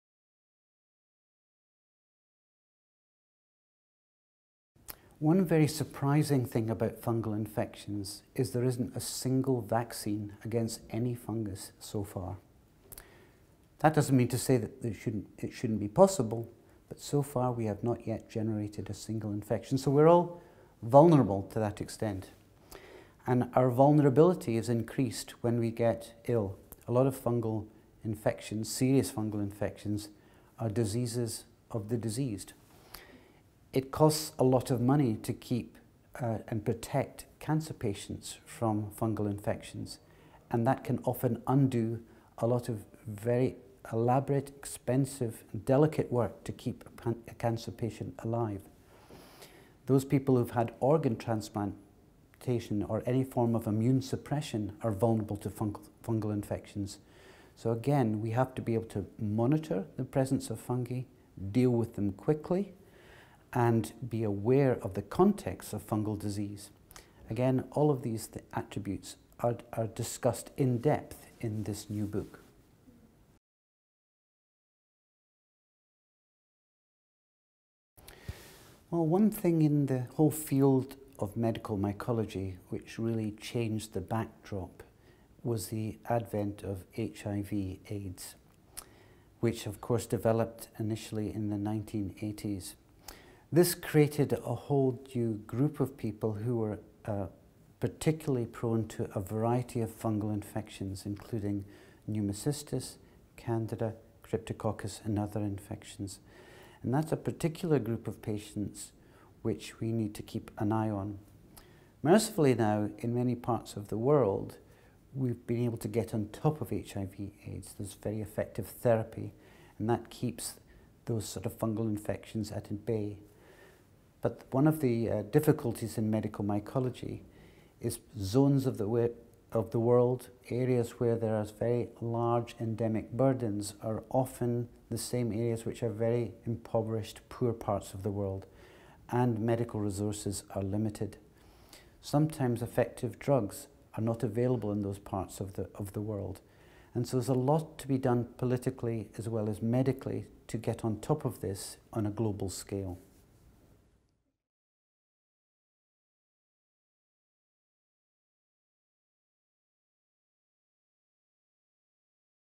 How Can We Prevent Fungal Infections? | Professor Neil A.R. Gow
Professor Neil A.R. Gow talks about fungal vaccines, and why HIV/AIDs patients are particularly vulnerable to infection.

Professor Gow is co-editor of the Oxford Textbook of Medical Mycology, which is a comprehensive reference text that brings together the science and medicine of human fungal disease.